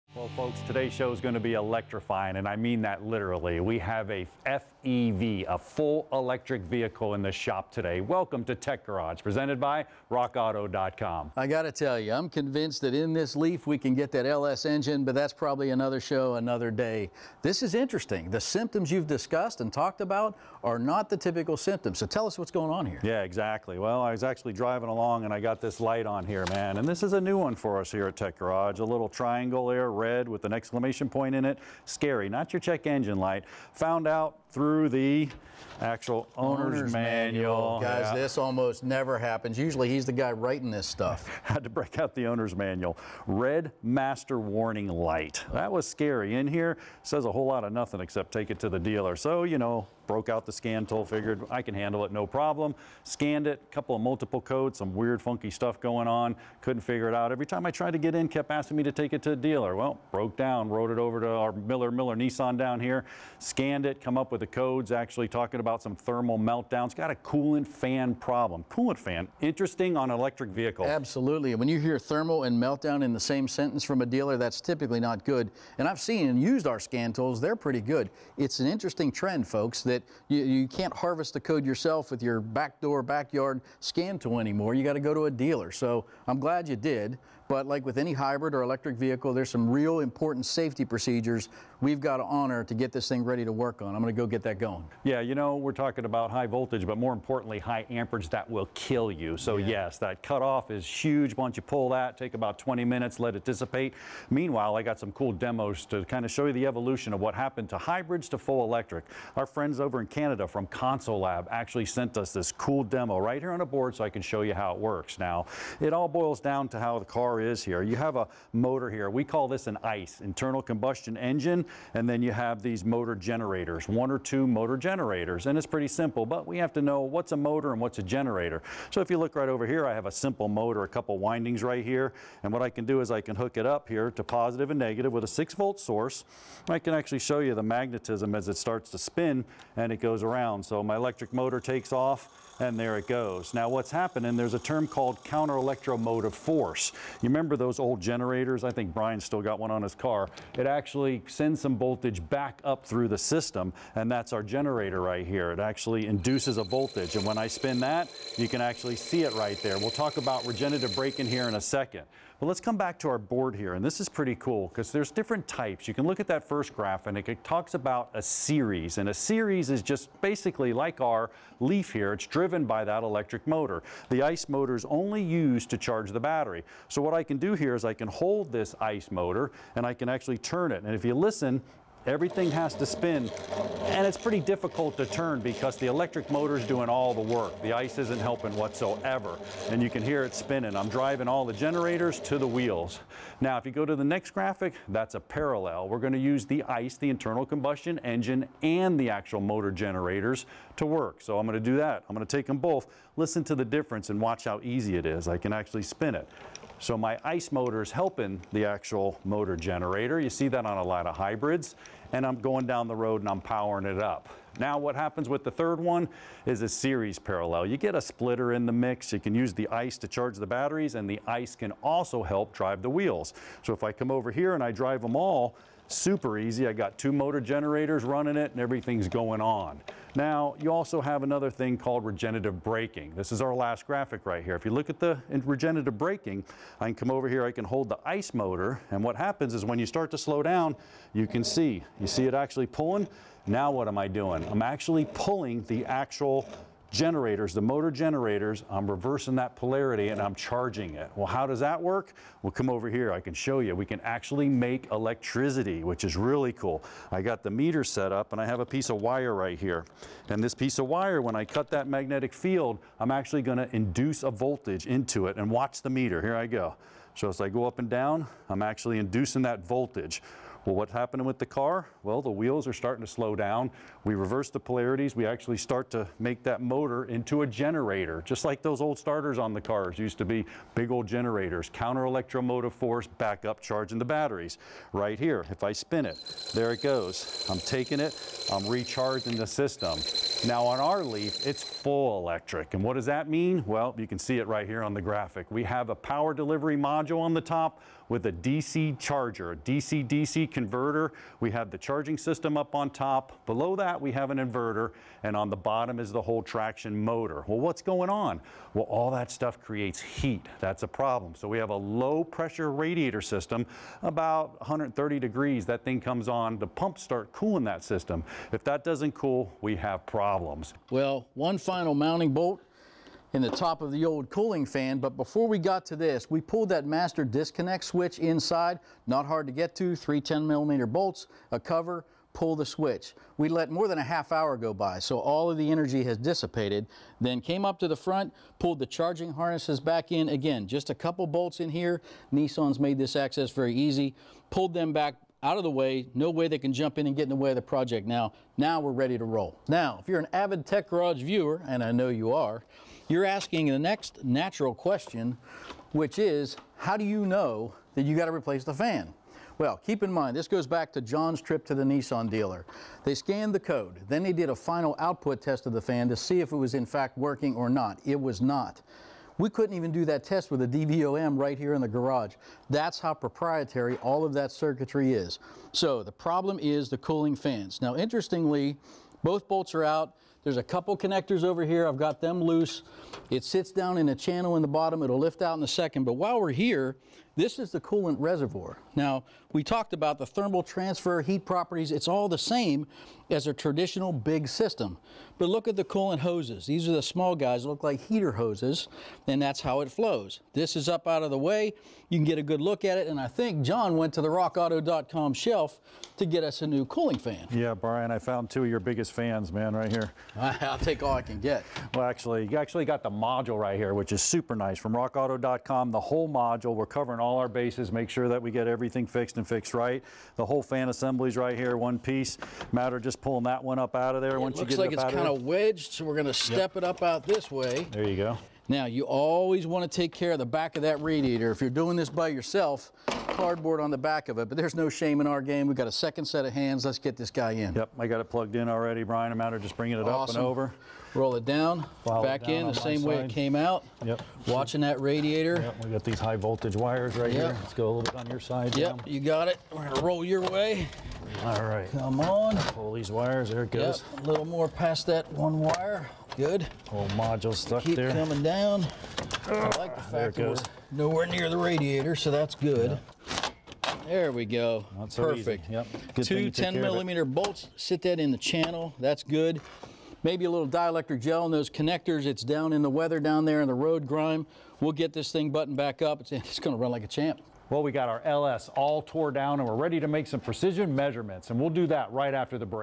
Show 8 Electric Vehicle - How It Works & Keeping It Cool
Amazingly "normal" cooling system on a Fully Electric Vehicle!  Who knew?  We dive in an demystify!